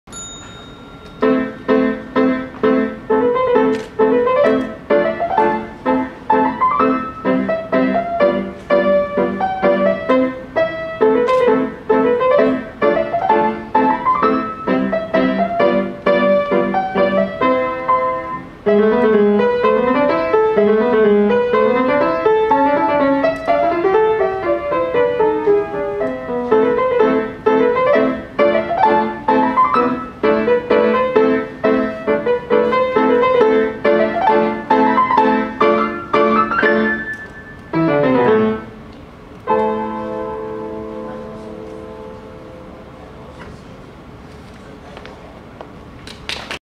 第五屆全港少年鋼琴精英賽 22 蘇軒彤 第三級 亞軍
第五屆全港少年鋼琴精英賽

日期: 16/8/2015
地點: 旺角綜合大樓會堂